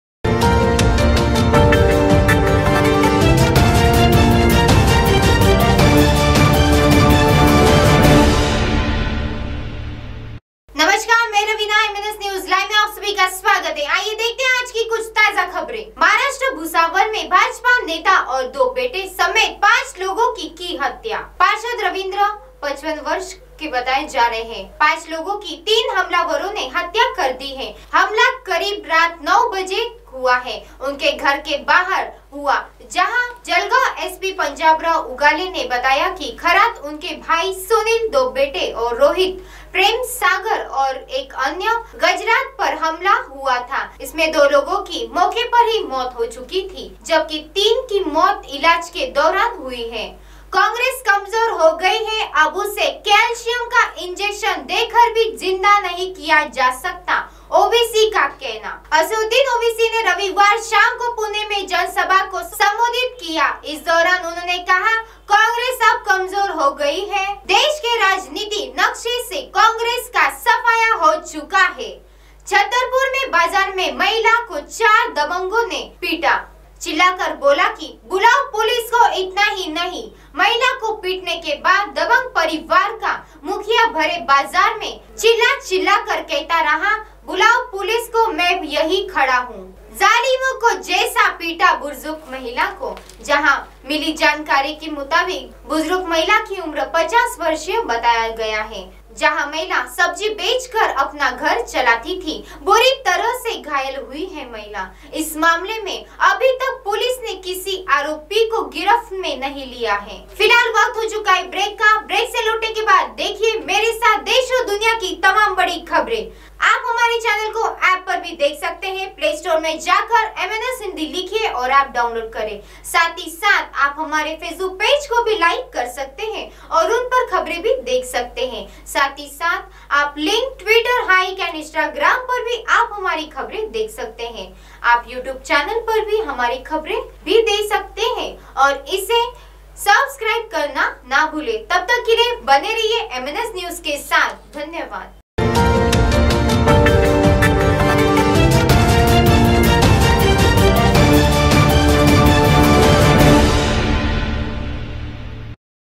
MNS औवेसी-कांग्रेस कमजोर हो गई है,अब उसे कैल्शियम का इंजेक्शन दे के भी जिन्दा नहीं किया जा सकता।
MNSNEWSLIVE औवेसी-कांग्रेस कमजोर हो गई है,अब उसे कैल्शियम का इंजेक्शन दे के भी जिन्दा नहीं किया जा सकता।